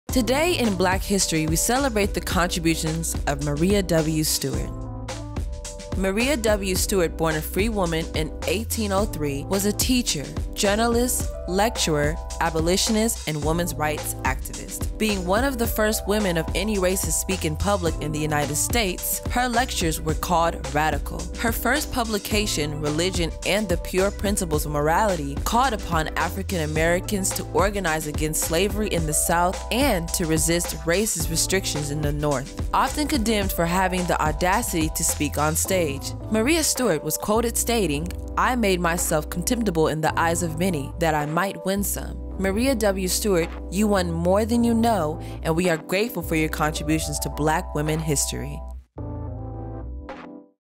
Buy From a Black Woman celebrates the contributions of Maria W. Stewart
Today in Black History we celebrate the Contributions
of Maria W. Stewart

Maria W. Stewart, born a free woman in 1803, was a teacher, journalist, lecturer, abolitionist, and women's rights activist. Being one of the first women of any race to speak in public in the United States, her lectures were called radical. Her first publication, Religion and the Pure Principles of Morality, called upon African Americans to organize against slavery in the South and to resist racist restrictions in the North. Often condemned for having the audacity to speak onstage, Maria Stewart was quoted stating “I made myself contemptible in the eyes of many, that I might win some,"

Maria W. Stewart, you won more than you know and we are grateful for your contributions to Black Women History.